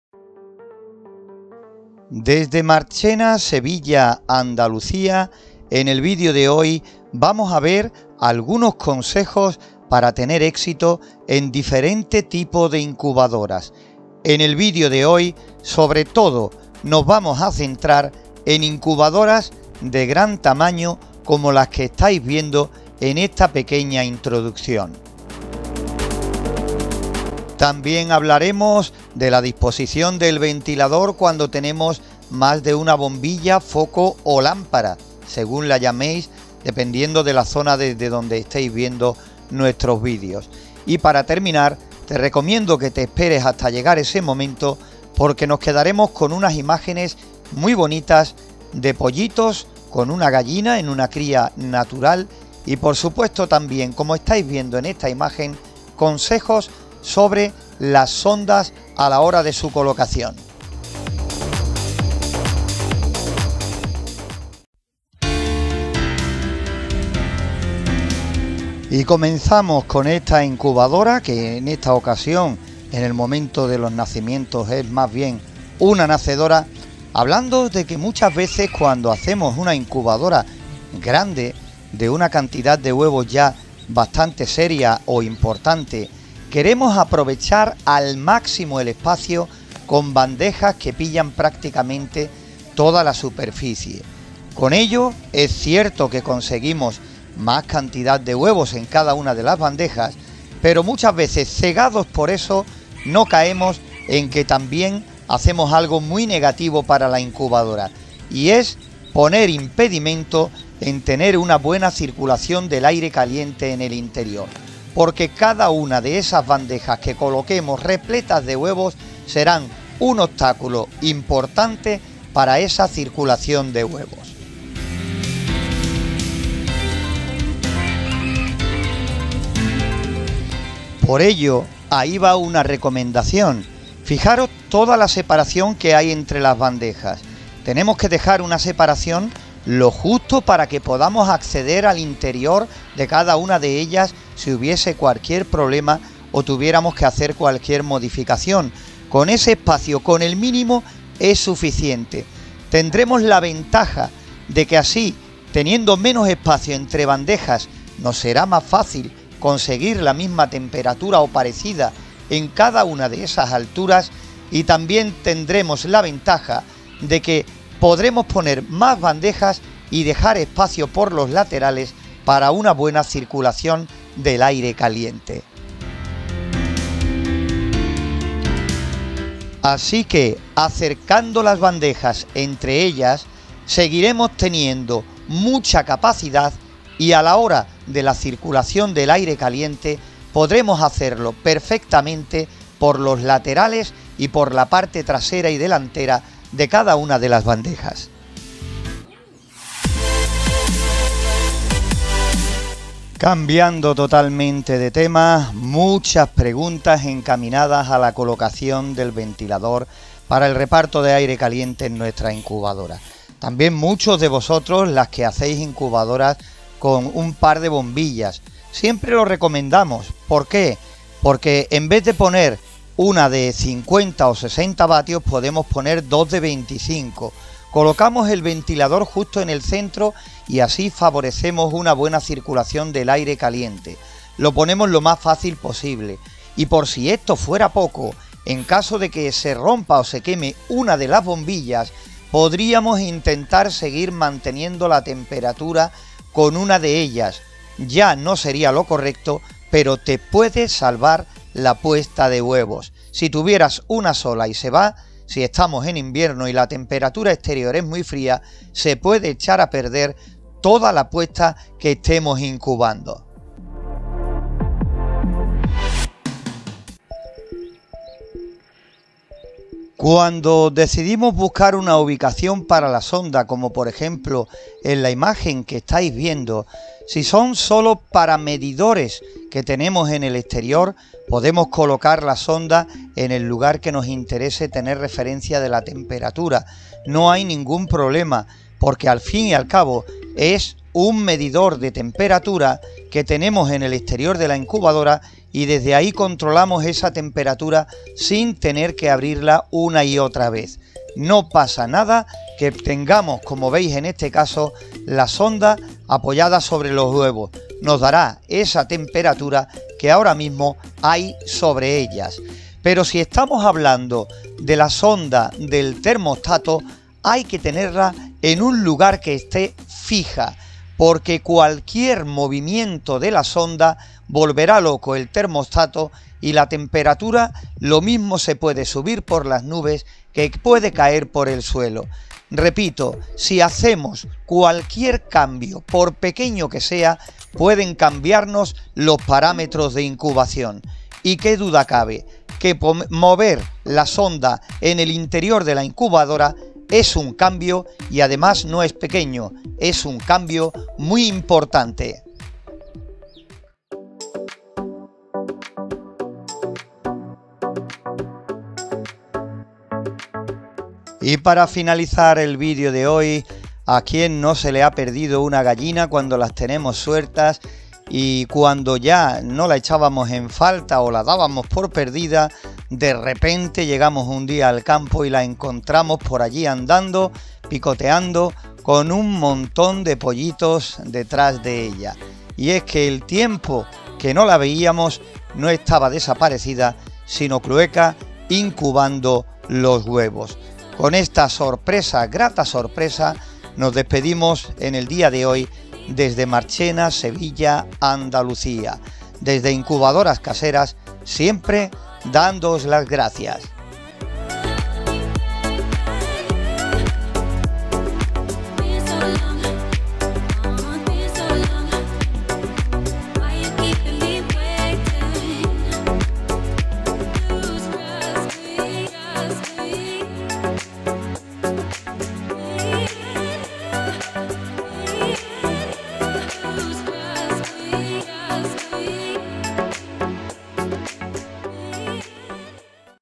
Cómo hacer una incubadora casera, CONSEJOS
Cómo hacer una incubadora casera fácil, económica, rápido y paso a paso con los diferentes consejos que vamos dando sobre incubación.
00:00 Introducción
01:05 Incubadora casera en un frigorífico con bombillas, focos o lámparas en plena incubación de huevos de gallinas con nacimiento de pollitos.
03:19 Cómo poner el ventilador o abanico en una incubadora casera para conseguir buenos resultados.
04:27 Dónde colocar la sonda en la incubadora, la del termostato o la del termómetro o medidores de humedad relativa.
04:56 Gallina ponedora clueca con pollitos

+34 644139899